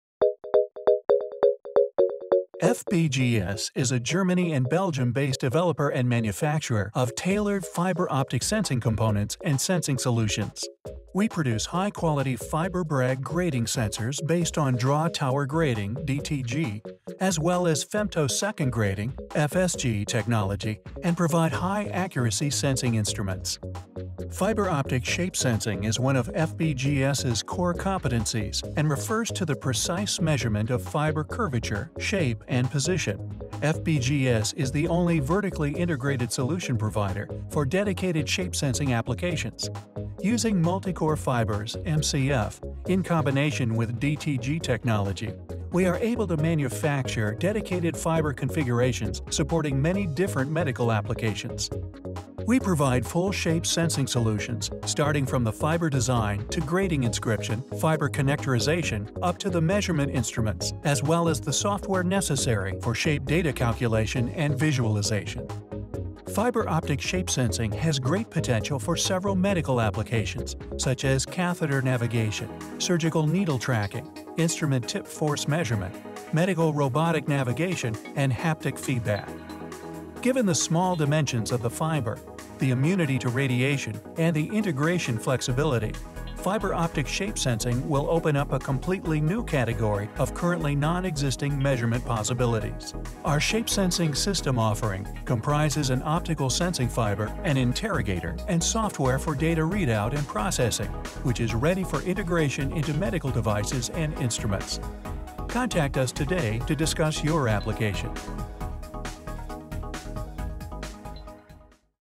Shape Sensing FBGS Tailored Fiber Optic Sensing Components and Solutions 2020
Fiber optic shape sensing is one of FBGS´s core competences and refers to the precise measurement of fiber curvature, shape and position. FBGS is the only vertically integrated solution provider for dedicated shape sensing applications. Using multicore fibers (MCF) in combination with DTG technology we are able to manufacture dedicated fiber configurations supporting many different medical applications. We provide full shape sensing solutions, starting from the fiber design, to grating inscription, fiber connectorization up to the measurement instruments as well as the software necessary for shape data calculation and visualization.
By processing the differential strain signals reflected by DTGs in multicore fibers with our own developed sensing algorithms we are able to reconstruct the curvature, shape and position of the fiber with mm accuracy. The associated software will provide shape data and coordinates of the fiber in 3D for further processing or direct visualization.

Fiber optic shape sensing has great potential for several medical applications such as catheter navigation, surgical needle tracking, instrument tip force measurement, medical robotic navigation and haptic feedback. Given the small dimensions of the fiber, the immunity to radiation and the integration flexibility, fiber optic shape sensing will open up a completely new category of currently non-existing measurement possibilities.
Our shape sensing system offering comprises an optical sensing fiber, an interrogator and software for data readout and processing which is ready for integration into medical devices and instruments.